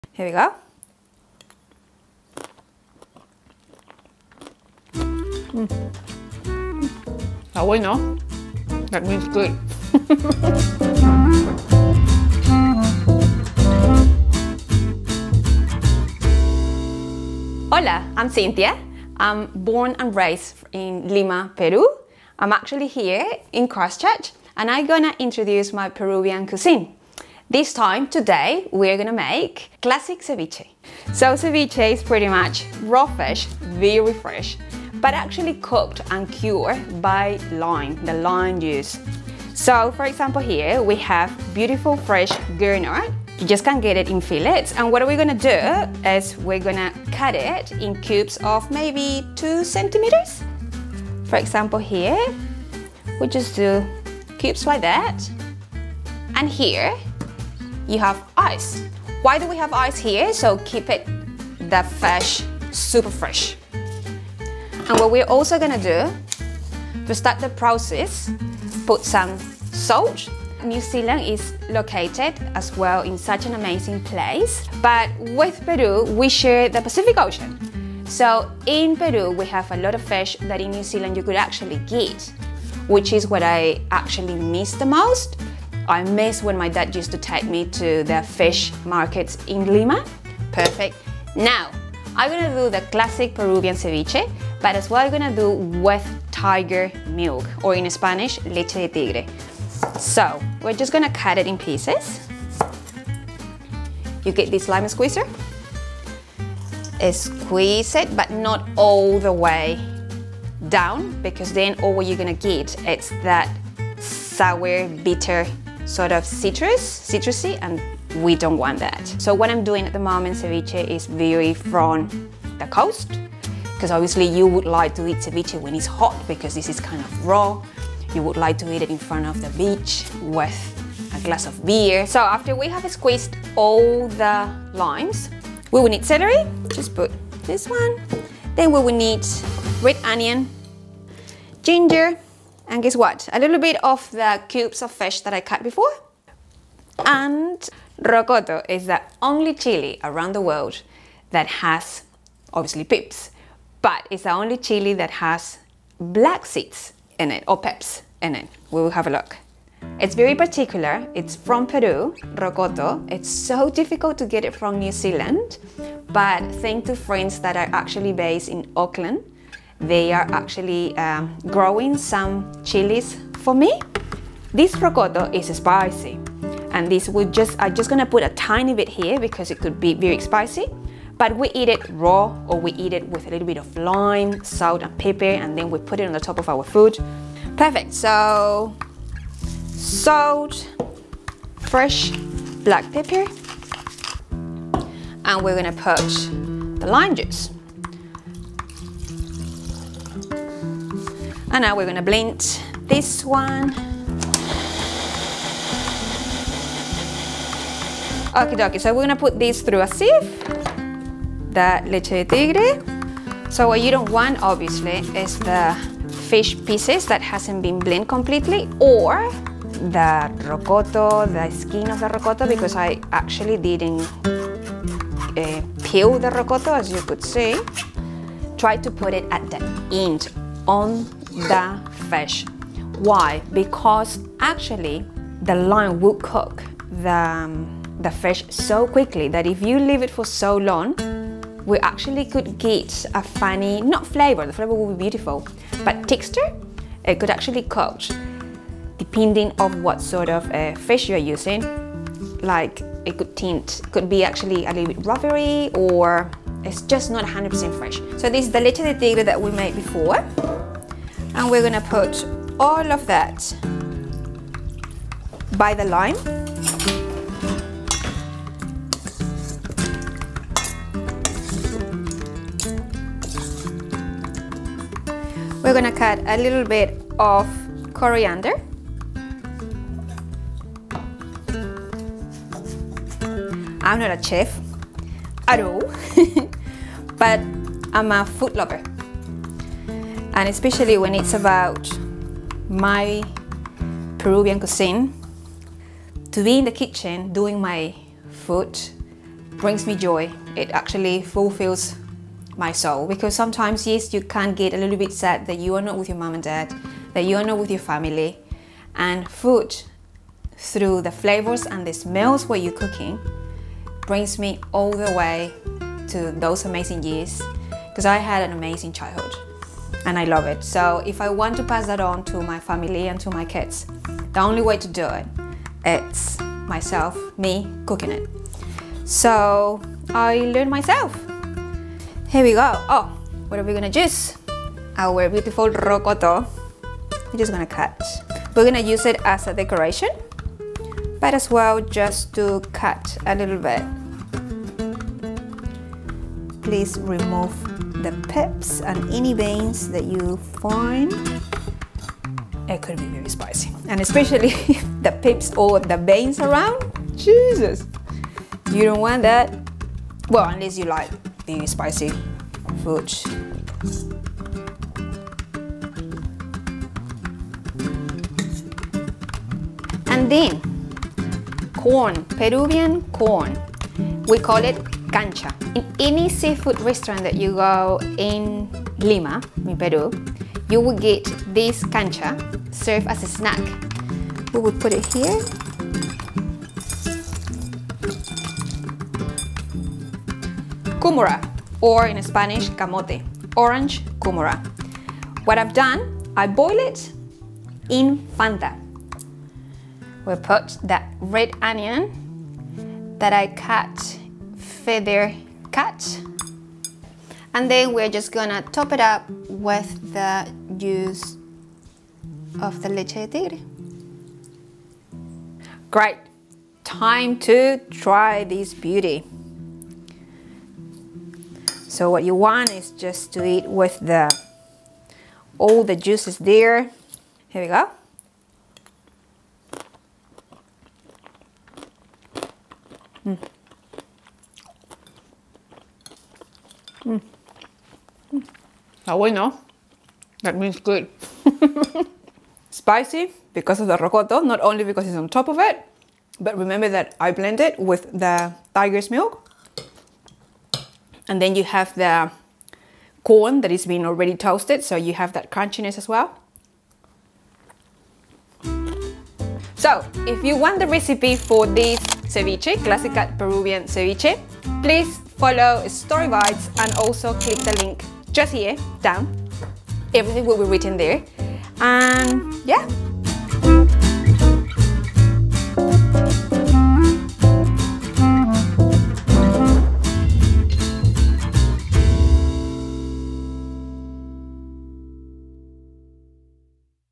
The Best Fresh Fish Dish You'll Make This Summer - Pervian Ceviche Recipe
The freshest ingredients make all the difference in this classic fish ceviche. Enjoy this famous dish of South America's most exciting culinary destination: Peru!

INGREDIENTS: For 2 portions

- 400 gr of Gurnard or Tarakihi
- 50 ml of freshly squeeze limes (4 limes)
- 1 red onion
- 1/2 cup of coriander
- 2 medium orange kumara
- 1 rocoto or red chili

Leche de Tigre Ingredients:
- 2 celery stalks
- 2 garlic clove
- 1 tsb fresh ginger
- 1/4 chopped onion
- 200 ml of freshly squeeze limes (12 units)
- 150 gr fish
- a splash of water or fish stock

To serve:
- Lettuce leave
- Chulpe Peruvian corn
- Boiled Kumara

Instructions:

Step 1: Wash and dry the fish. Remove any remaining skin, scales or spines.Cut the fish into cubes of approximately 2 cm. Put the fish in a bowl and this bowl on top of another one full of ice to keep it fresh. Add a bit of salt and refrigerate.

Step 2: Wash the rocoto (or red chilli) and red onion. Open the rocoto, remove the seeds and veins and cut into small squares. Peel the onion and cut it into thin strips (feather cut). Finely cut the coriander.

Step 3: Peel the kumara, cut in slices and boil it.

Leche de Tigre Instructions:

Step 1: Blend all ingredients and pass it through a sieve. Reserve.

Ceviche final instructions and presentation:

Step 1: Get the bowl with fish from the fridge and add the 50 ml lime juice. Mix well.

Step 2: Add the finely cut rocoto, coriander and feather cut onion.

Step 3: Add the Leche de Tigre and keep mixing. At this stage have a taste and add more salt if needed.

Step 4: In a bowl or plate add the ceviche on top of crispy lettuce leaf with the help of a spoon. Add the boiled kumara in slices next to the ceviche, top it up with cancha (Peruvian corn) and coriander. Eat immediately as the lime has been curating the fish already.

Tip 1: Cut the onion in thin strips or feather cut and add cold water with 2-3 ice cubes a bit of salt. This will keep the onion crunchy and will remove its bitterness.

Tip 2: Boil the kumara (sweet potato) in Fanta! Yes, Fanta!!! Alternatively, boil it in orange juice + 2 tbs of sugar and star aniseed. This will enhance the kumara flavour and give it a nicer colour.

Tip 3: You can skip the Step 2 of the Final Ceviche plating. Instead, add the rocoto (or red chillies), chopped coriander and onion at the end. Make the presentation of this dish unique and your signature!

Tip 4: Adding salt to the fish already cut in squares will start curating the fish.

Tip 5: The bowl of ice will not only keep the fish fresh but also firm.

How do I normally eat ceviche in Peru?

At the beach, with friends or family and a very cold beer. Ceviche means party, summer, waves, music and people, your people!!